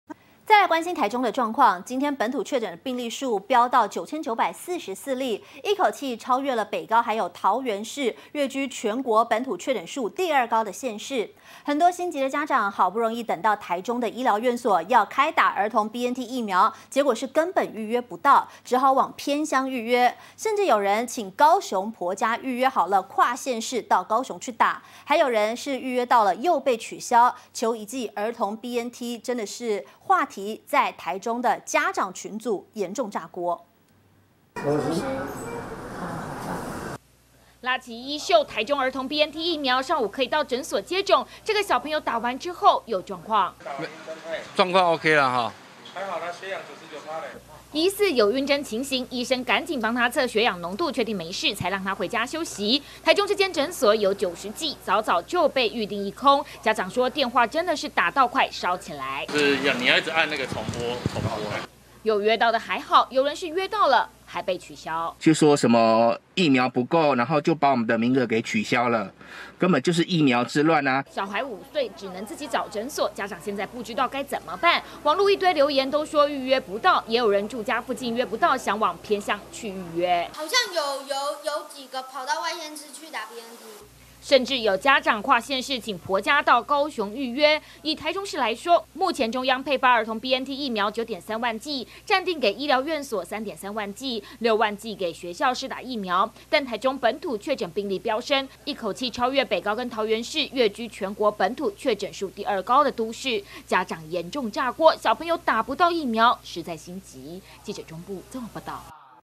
兒童BNT開打!孩童疑暈針 醫急測血氧濃度
台中本土確診病例飆升到9944，逼近萬人，一口氣超越北高跟桃園市，越居全國本土確診數第二高的都市.心急的家長好不容易等到台中市醫療院所開打兒童BNT疫苗，結果根本預約不到，只好往偏鄉預約，甚至有人直接請高雄婆家預約跨縣市到高雄打，還有人是預約到又被取消，「求一劑量兒童BNT疫苗」話題在台中市家長群組嚴重炸鍋！